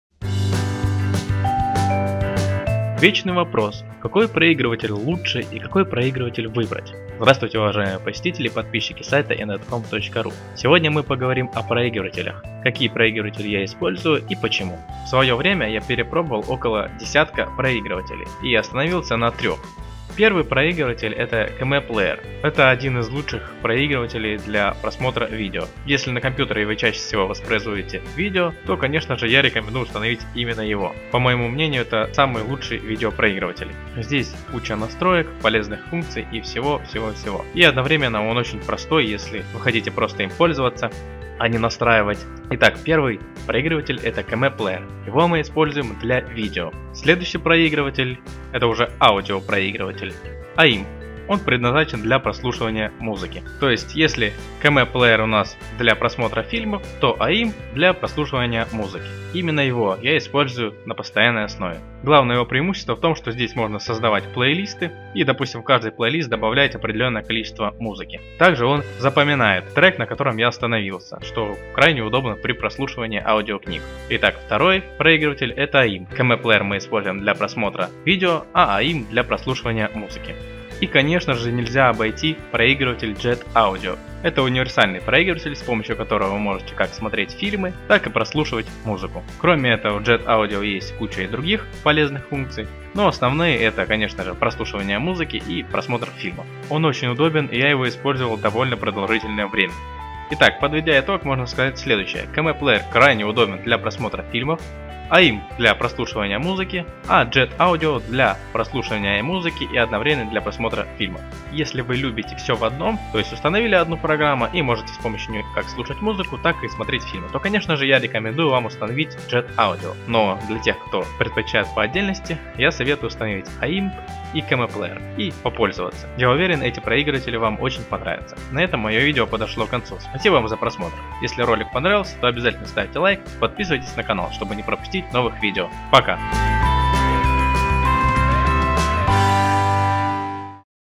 Какой проигрыватель для компьютера выбрать? Мой совет!
Сегодня я попробую ответить на следующий вопрос: Какой проигрыватель выбрать? Я в своё время испробовал около десятка различных проигрывателей и пришёл к выводу, что очень хороших проигрывателей для музыки и видео не так то много.

Какой проигрыватель выбрать если вы хотите только слушать музыку? Ответ в видео! Какой проигрыватель выбрать если вы хотите только воспроизводить видео? Ответь в видео! Какой проигрыватель выбрать если вы хотите и музыку слушать и фильмы смотреть? Ответ в видео!